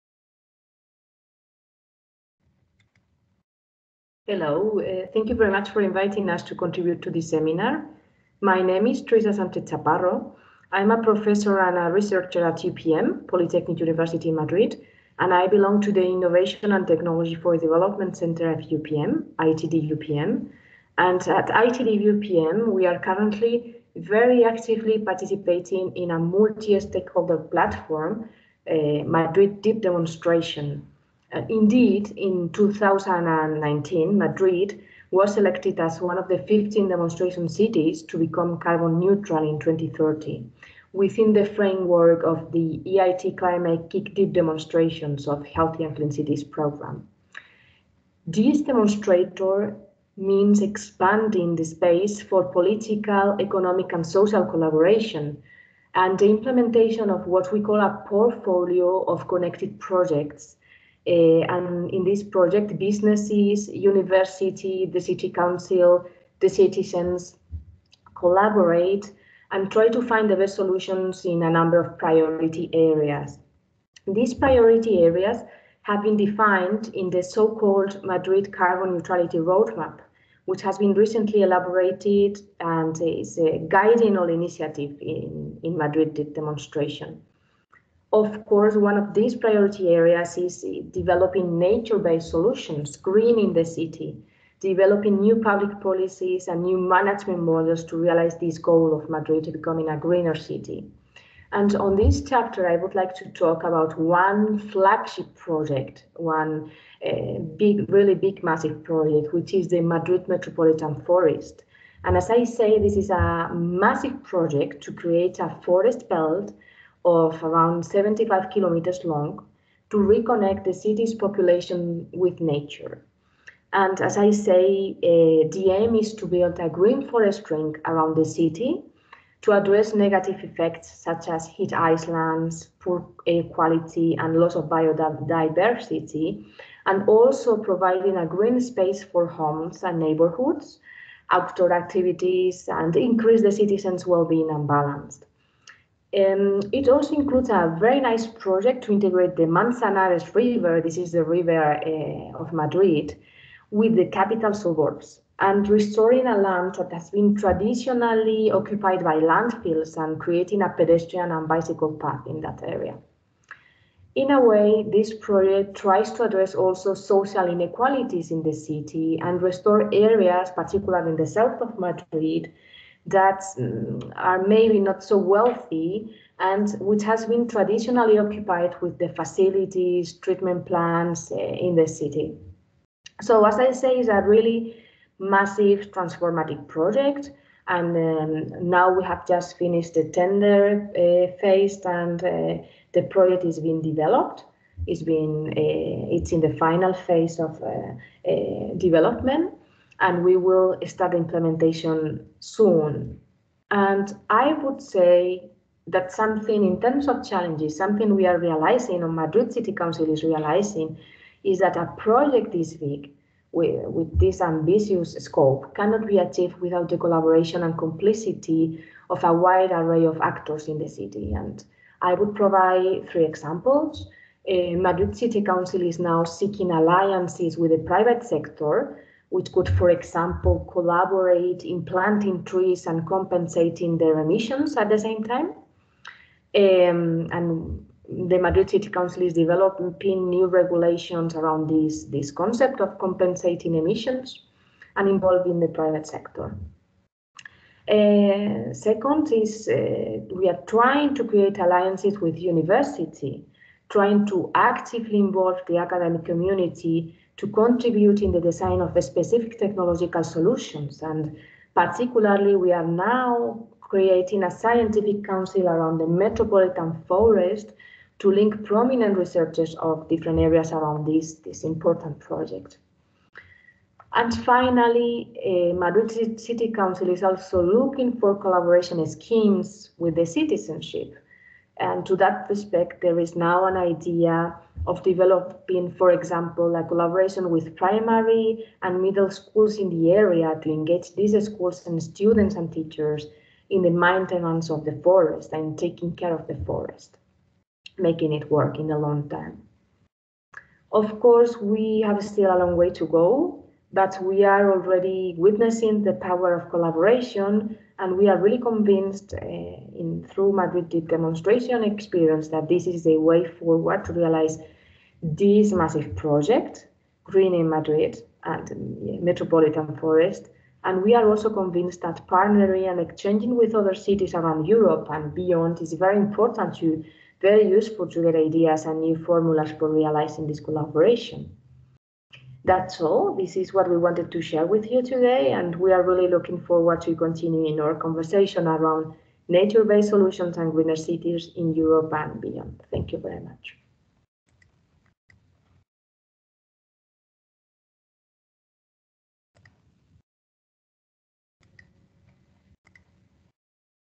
Going Green #02: Learning from Madrid
Teresa Sánchez Chaparro, scientific director, Innovation and Technology for Development Centre, Universidad Politécnica de Madrid.

In the context of 'Going Green', a live broadcast series on the theme 'Growing Green Cities' (Floriade Expo 2022), that we organize in collaboration with Province of Flevoland, we have the chance of sharing extra material with you from our knowledge network. This video is an addition to our session on July 1st, about the role of international collaboration in the transition to Green 'Making Green Work'. Enjoy and share!